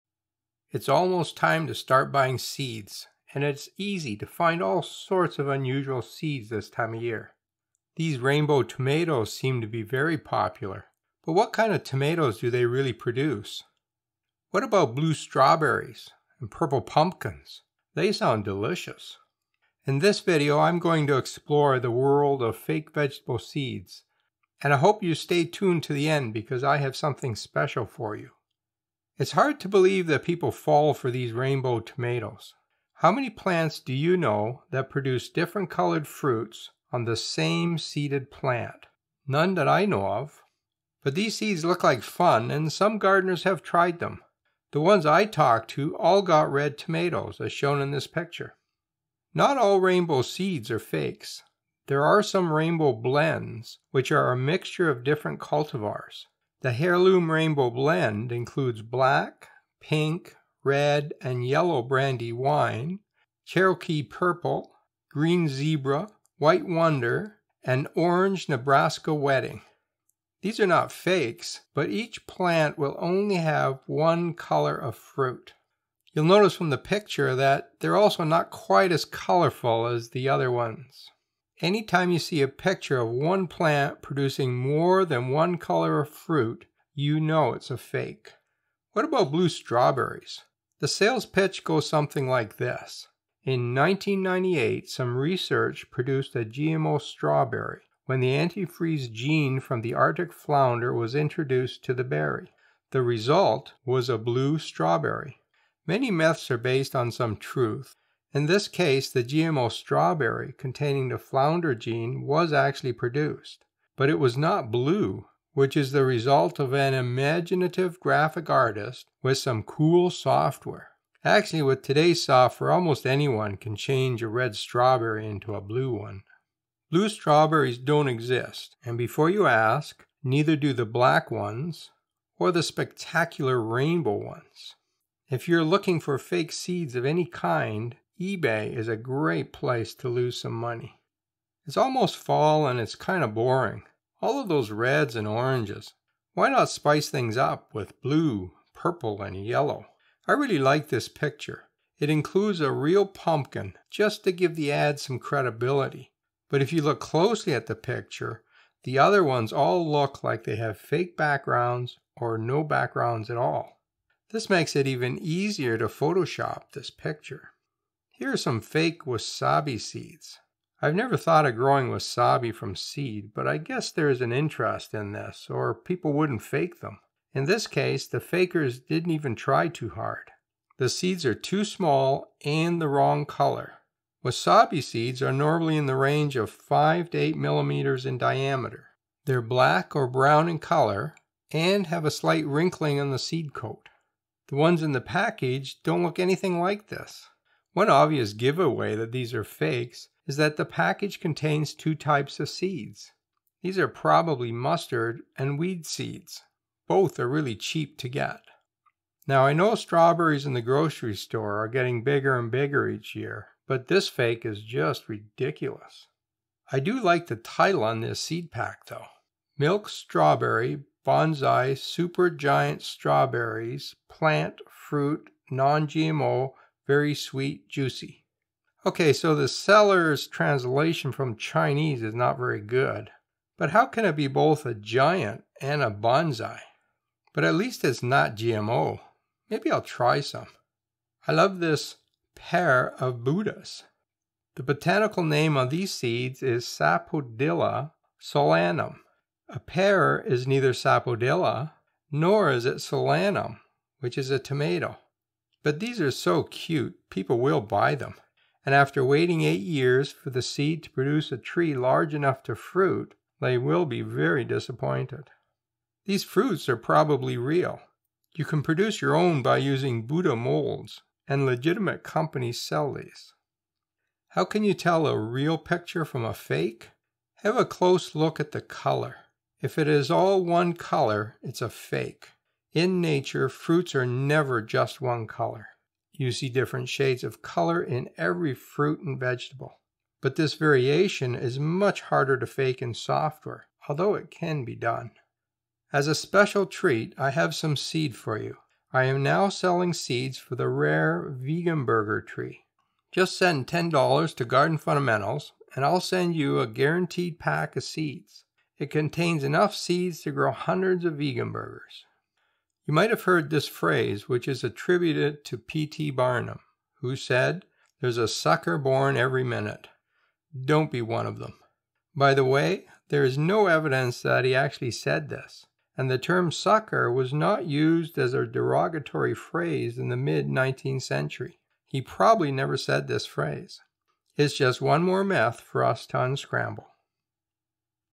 Fake Fruit and Vegetable Seed 🍎🥬️🍓🍇 Which Ones are Fake and How Do You Spot Them?
Don't get fooled by fake seeds. Learn how to recognize fake fruit and vegetable seed.

Blue strawberries, rainbow tomatoes, blue pumpkins, pear shaped buddhas